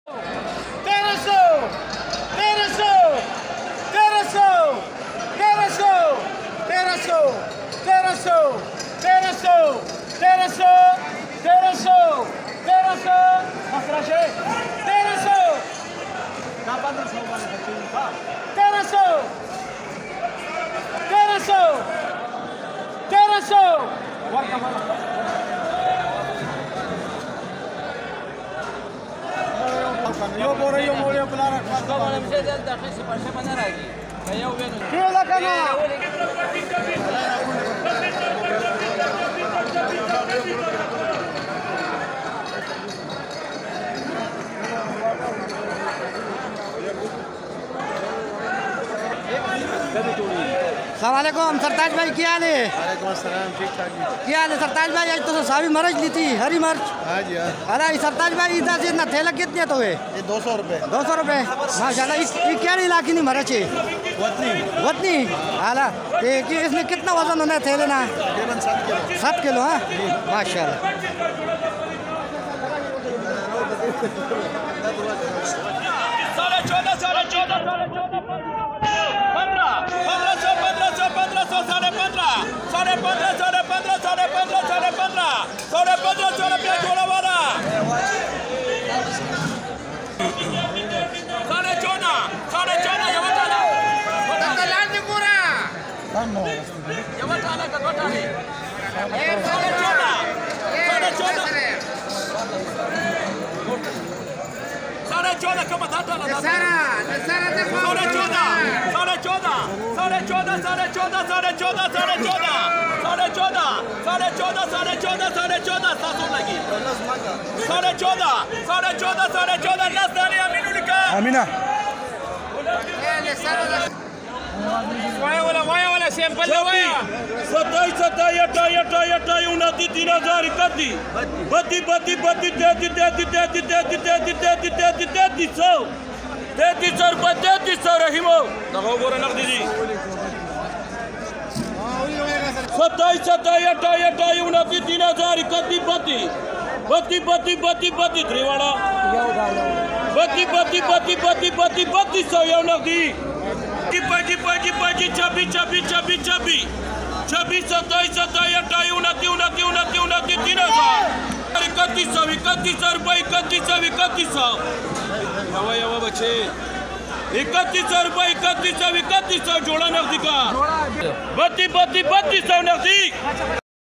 سبزی منڈی پشاور" آج کی تازہ ترین، مقامی ہری مرچ اور کمالیہ بھنڈی اپڈیٹ
Bhindi, commonly known as okra or ladyfinger, is a vibrant and versatile vegetable that is highly sought after in the bustling Sabzimandi of Peshawar. With its slender, elongated shape and a vivid green hue, bhindi stands out as a visually appealing delicacy. Its tender texture and mild taste make it a popular choice for a wide range of culinary creations.

The bhindi on display during the hari mirch nilaami, or auction, at Sabzimandi Peshawar exemplifies the epitome of freshness. Glistening with dewdrops, these carefully handpicked pods exude a natural vibrancy that reflects their peak quality. The smooth, unblemished surface of each bhindi is a testament to the meticulous care taken in its cultivation and selection.

Paired alongside the hari mirch, or green chili, the nilaami showcases an enticing combination of flavors and colors. The hari mirch, with its lively green shade and slender form, adds a distinct element of spiciness and zing to the auction. This vibrant chili complements the bhindi perfectly, providing a tantalizing contrast of tastes that stimulates the palate.

As the auction proceeds, the aroma of freshly harvested bhindi and the pungent fragrance of the hari mirch fill the air, creating an enticing atmosphere. Buyers eagerly assess each lot, evaluating the bhindi for its crispness, vibrant color, and ideal size, while the hari mirch is scrutinized for its heat level and overall freshness.

The bhindi and hari mirch nilaami at Sabzimandi Peshawar is a captivating spectacle that showcases the richness and diversity of the local produce. It serves as a testament to the region's agricultural prowess and the dedication of farmers who cultivate these exquisite vegetables.